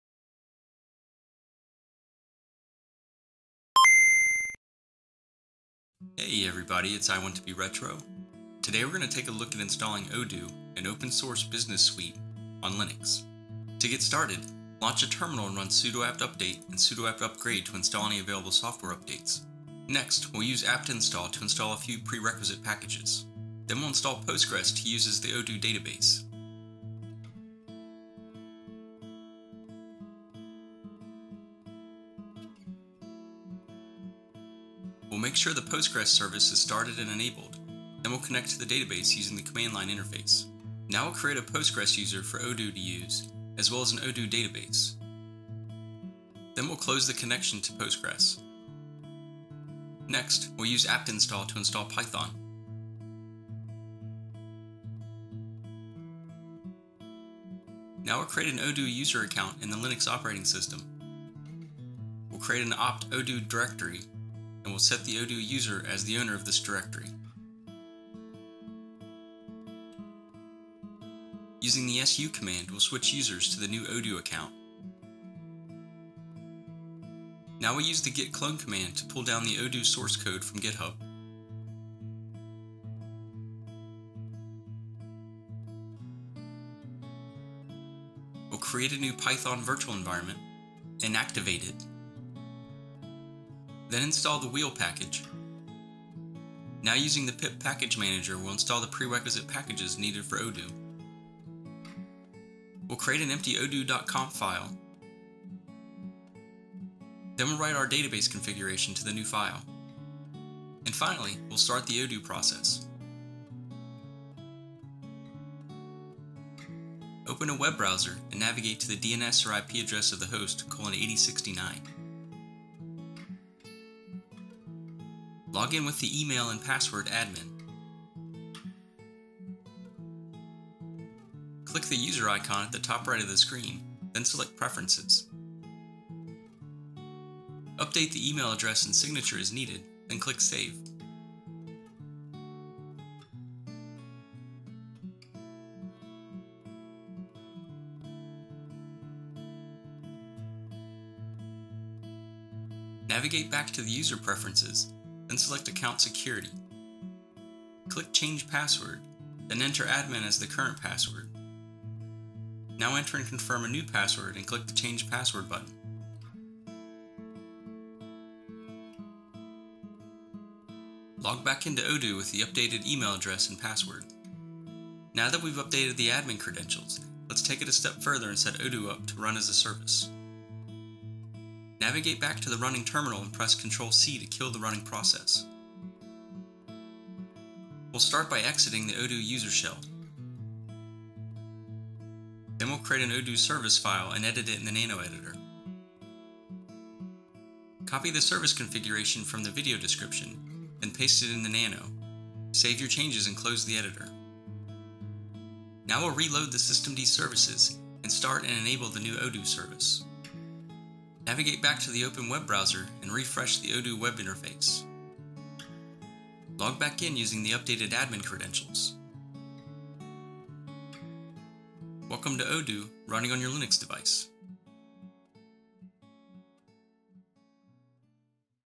Install Odoo - Open Source Business Application Suite - on Linux
What is Odoo?
Odoo is a suite of web based open source business apps. The main Odoo Apps include an Open Source CRM, Website Builder, eCommerce, Warehouse Management, Project Management, Billing & Accounting, Point of Sale, Human Resources, Marketing, Manufacturing. Odoo Apps can be used as stand-alone applications, but they also integrate seamlessly so you get a full-featured Open Source ERP when you install several Apps.

Installation
   01. Log into the Linux device
   02. Run the following commands in terminal
         # update software repositories
         sudo apt update
         # install available software updates
         sudo apt upgrade
         # install prerequisites
         sudo apt install wget git apt-transport-https -y
         # install postgresql
         sudo apt install postgresql postgresql-client -y
         # enable the postgresql service and start it
         sudo systemctl enable postgresql --now
         # connect to postgresql
         sudo -u postgres psql postgres
         # create odoo database user
         create user odoo_rw with password '0dooDB_rw$';
         # create odoo database
         create database odoo with encoding='UTF8' template='template0' owner='odoo_rw';
         # close postgresql connection
         exit
         # install python
         sudo apt install python3-full python3-pip libldap2-dev libpq-dev libsasl2-dev -y
         # create odoo user
         sudo useradd -M odoo
         # create /opt/odoo directory
         sudo mkdir /opt/odoo -p
         # set owner of /opt/odoo
         sudo chown odoo /opt/odoo -R && sudo chgrp odoo /opt/odoo -R && sudo usermod -d /opt/odoo odoo
         # switch to odoo user
         sudo su - odoo
         # clone odoo from github
         # prepare the working directory
         python3 -m venv odoo-venv
         . odoo-venv/bin/activate
         # install wheel
         pip3 install wheel
         # install odoo
         pip install -r requirements.txt
         # create a config file
         touch ./odoo.conf
         # write database config to config file
         echo "[options]\ndb_user = odoo_rw\ndb_password = 0dooDB_rw$\ndb_name = odoo\ndb_host = localhost" ≫ ./odoo.conf
         # run odoo
         ./odoo-bin --config ./odoo.conf -i base
03.
   04. Login with the username admin and password admin
   05. Click the user icon at the top right corner of the screen ≫ Preferences
   06. Update the Email and Email Signature ≫ Click Save
   07. Click the Account Security tab ≫ Click the Change Password button
   08. Enter admin as the current password ≫ Click Confirm Password
   09. Enter and confirm a new password ≫ Click Change Password
   11. Welcome to Odoo

Running Odoo as a Service
   01. Back in the Terminal, press CTRL+C to kill the running Odoo process
   02. Continue with the following commands in terminal
         # exit the odoo user shell
         exit
         # create odoo service file
         sudo nano /etc/systemd/system/odoo.service
   03. Paste the following into odoo.service
         [Unit]
         Description=Odoo
         Requires=postgresql.service
         After=network.target postgresql.service
         [Service]
         User=odoo
         Group=odoo
         ExecStart=/opt/odoo/odoo-venv/bin/python3 /opt/odoo/odoo-bin -c /opt/odoo/odoo.conf
         [Install]
         WantedBy=multi-user.target
   04. Press CTRL+O, Enter, CTRL+X to write the changes
   05. Continue with the following commands
         # reload systemd services
         sudo systemctl daemon-reload
         # start odoo service on boot and now
         sudo systemctl enable odoo --now
   06. Back in the web browser, refresh the Odoo tab